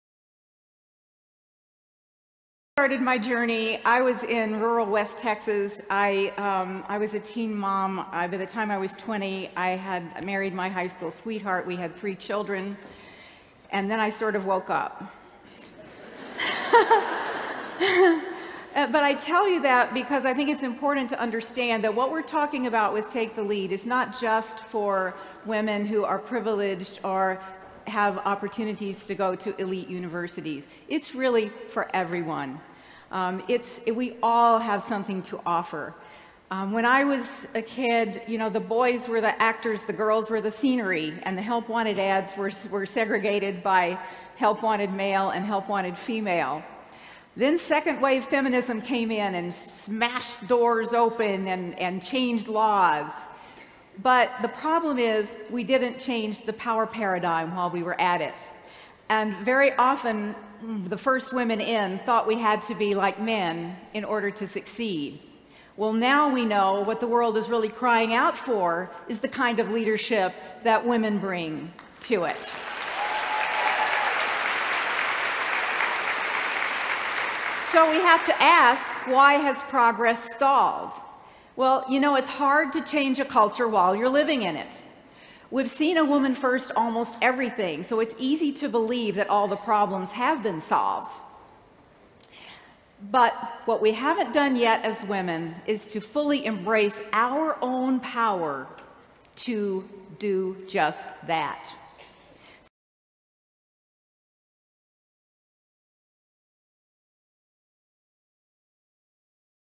Gloria Feldt: Embrace Our Power As Women
Take The Lead President Gloria Feldt talks about the women's movement and the next step we all must take. Filmed at the Take The Lead Challenge at Arizona State University on February 19, 2014.